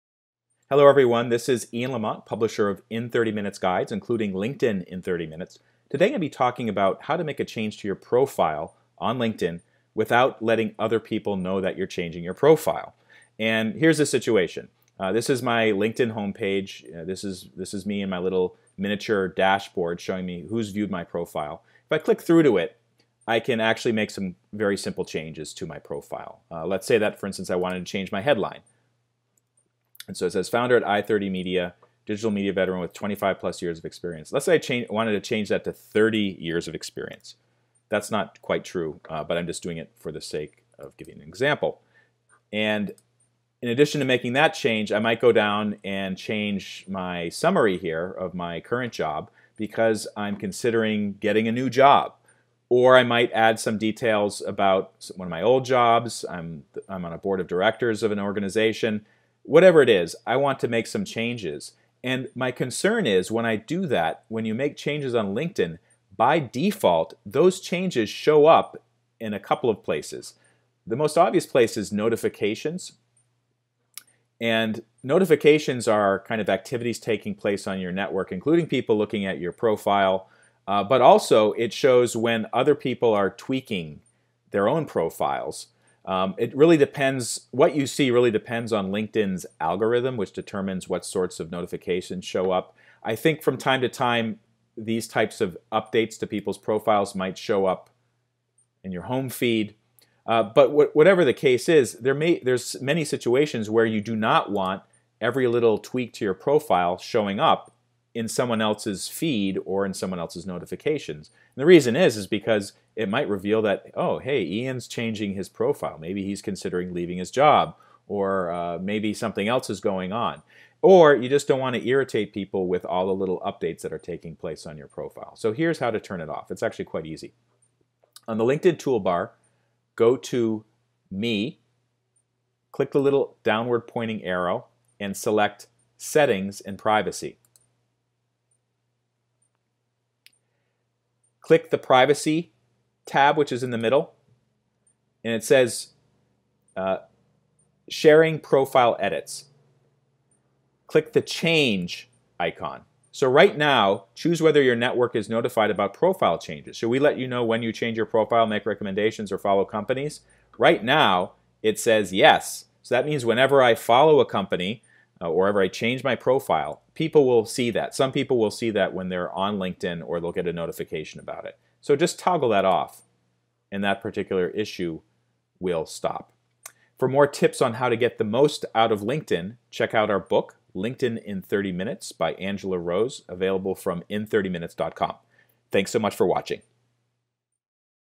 Make LinkedIn profile changes without notifying your network
It’s an age-old question: How to make LinkedIn profile changes without notifying your network. Maybe you are getting ready to change jobs and don’t want to tip off your boss by cleaning up your LinkedIn resume, or perhaps there are some changes to your current job description that you don’t want others to notice. Regardless, there’s an easy way to shut off profile updates, and this short video will show you how. Narrated by the publisher of LinkedIn In 30 Minutes.